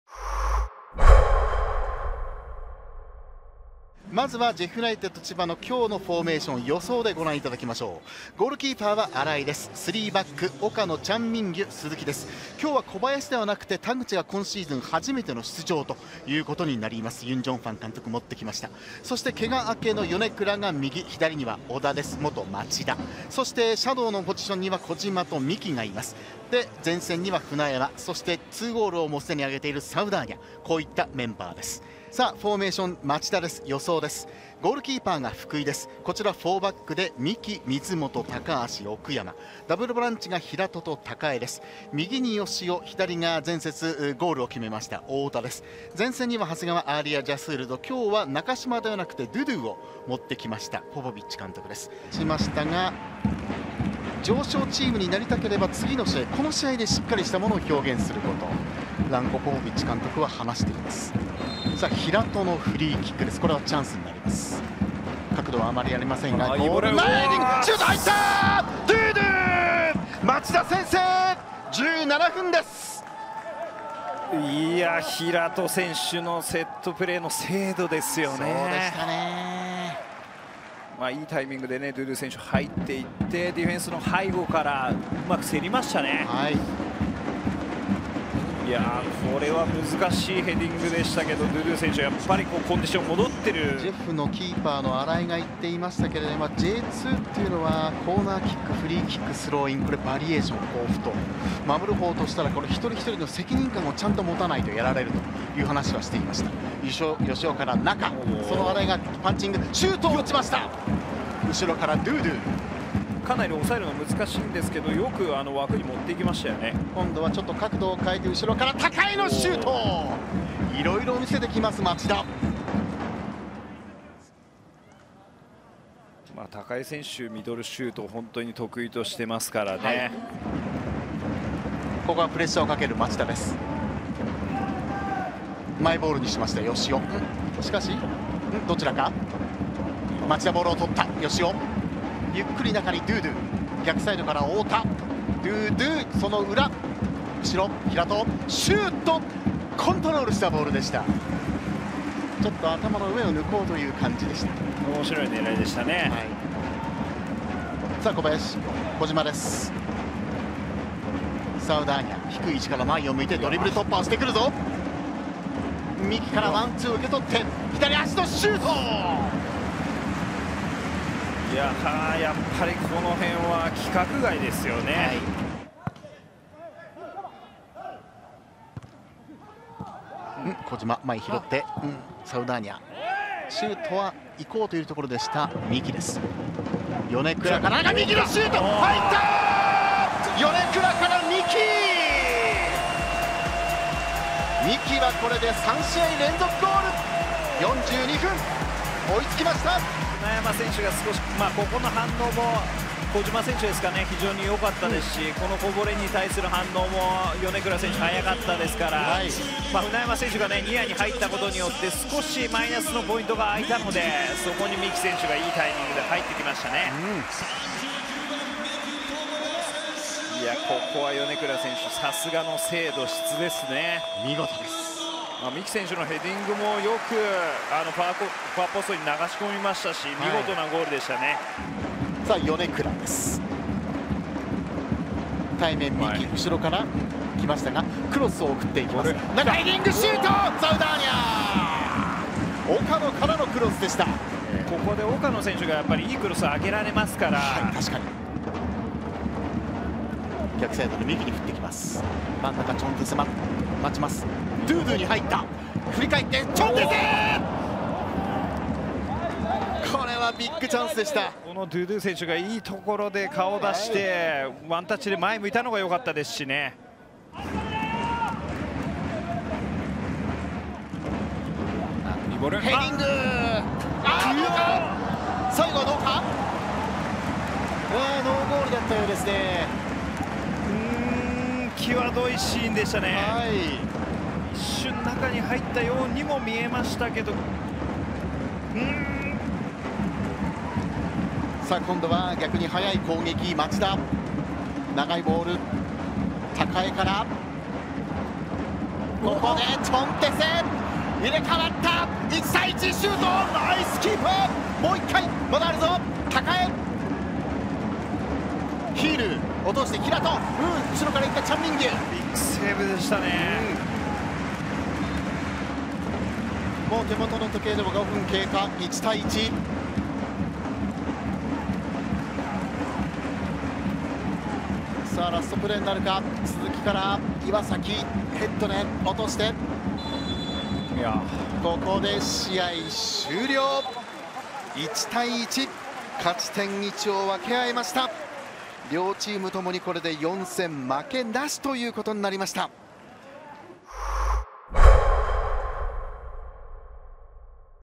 【ハイライト】FC町田ゼルビア vs ジェフユナイテッド千葉【明治安田生命J2リーグ第15節 2021.5.23】
【2021明治安田生命J2リーグ第15節 ハイライト動画】

0:00 両チームスタメン紹介
0:53 1-0 前半17分 ドゥドゥ(町田)
2:57 1-1 前半42分 見木友哉(千葉)
5:30 1-1 試合終了

福井光輝
奥山政幸
三鬼海
水本裕貴
高橋祥平
髙江麗央
平戸太貴
吉尾海夏
長谷川アーリアジャスール
太田修介
ドゥドゥ

▼サブ
茂木秀
深津康太
酒井隆介
奈良坂巧
岡田優希
鄭大世
中島裕希

▼監督
ランコ ポポヴィッチ

2021年5月23日（日）14時キックオフ
会場：フクダ電子アリーナ